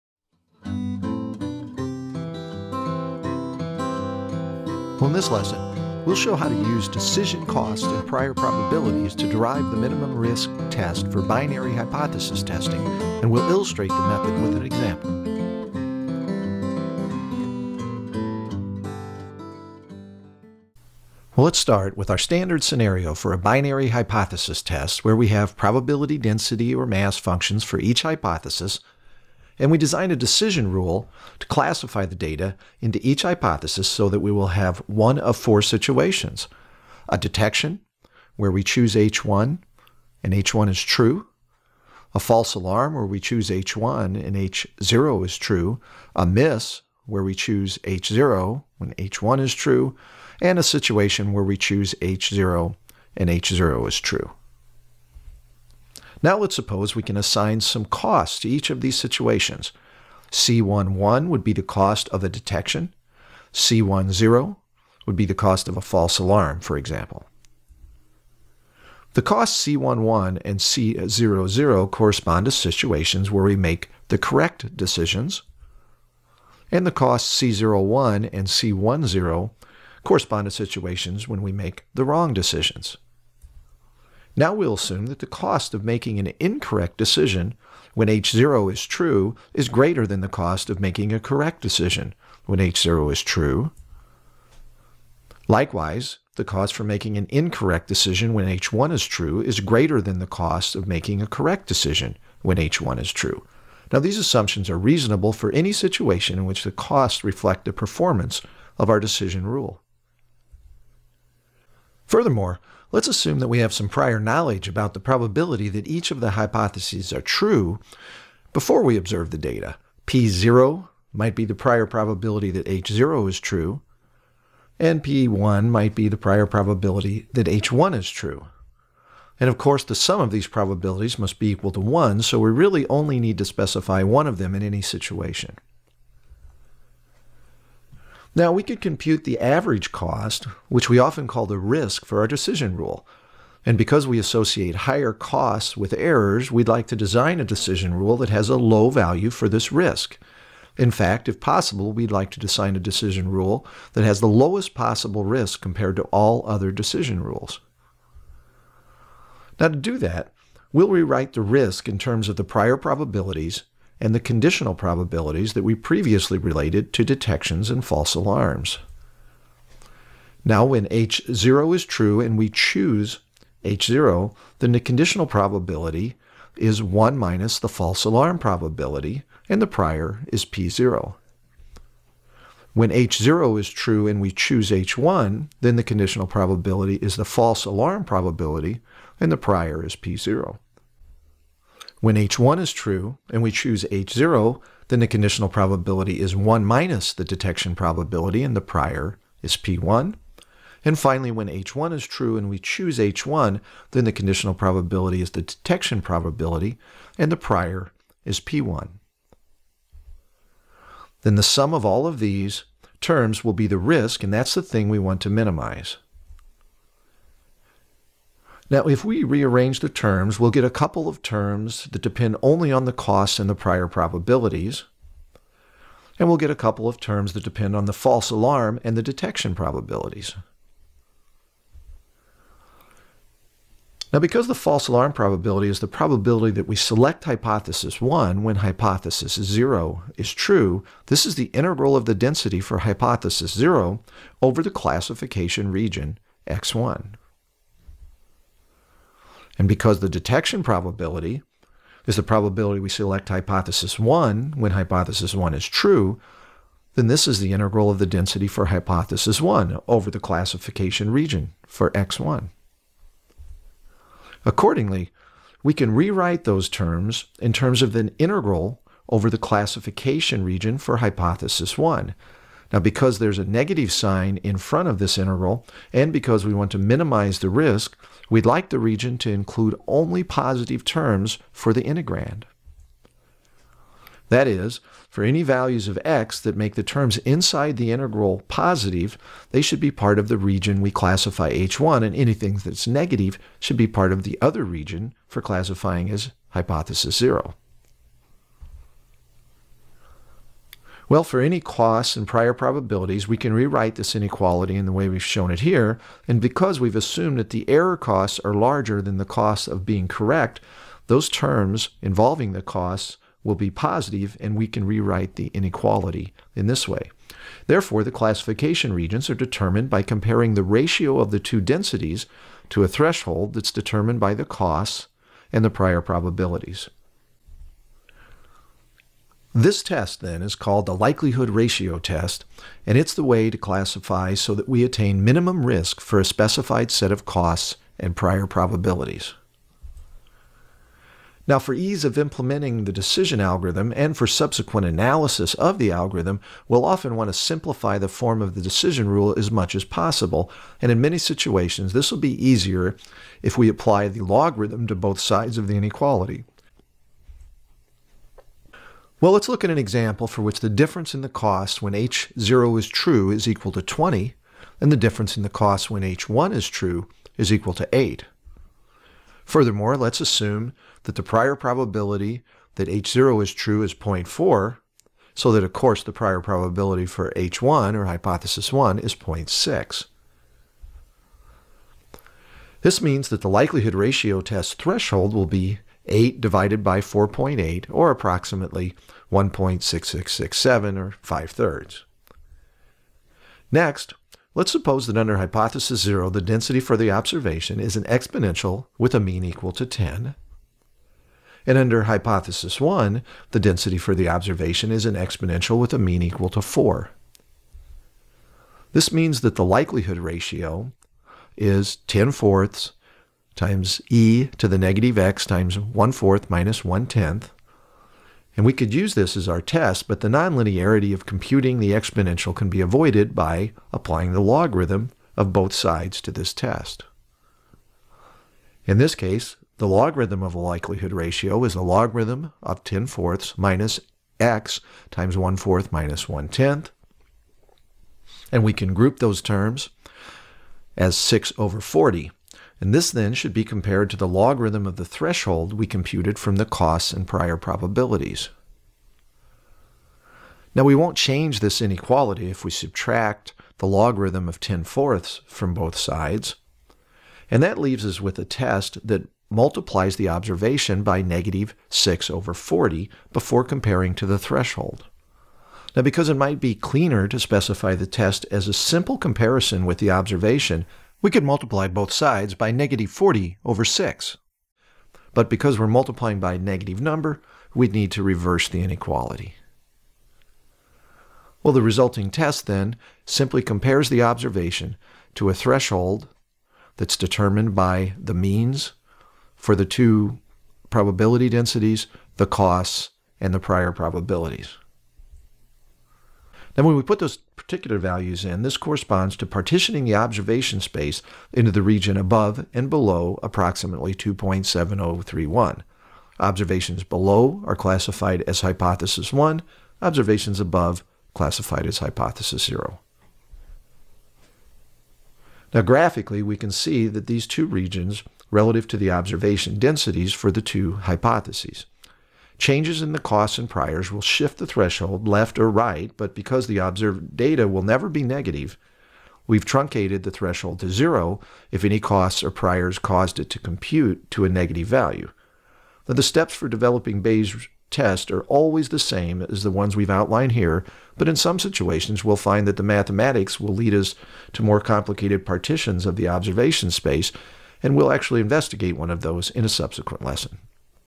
Bayes' Test for Binary Hypothesis Testing
In this lesson, we’ll show how to use decision costs and prior probabilities to derive the minimum risk test for binary hypothesis testing, and we’ll illustrate the method with an example.